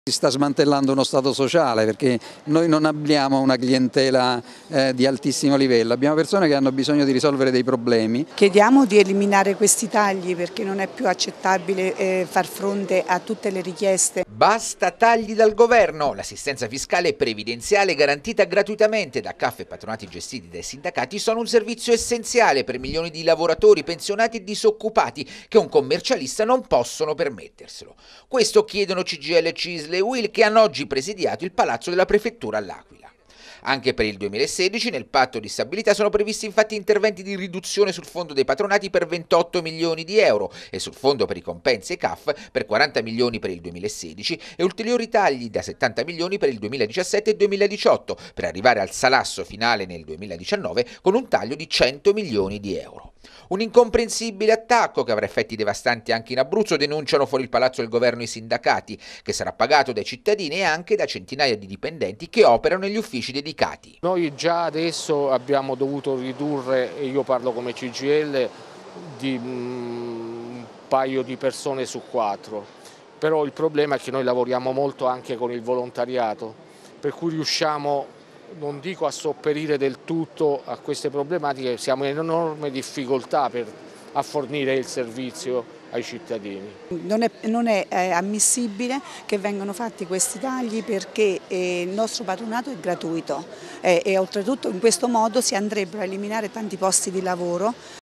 Tagli ai Caf, i sindacati manifestano a L'Aquila
Questa mattina a L'Aquila manifestazione unitaria di Cgil, Cisl e Uil contro i tagli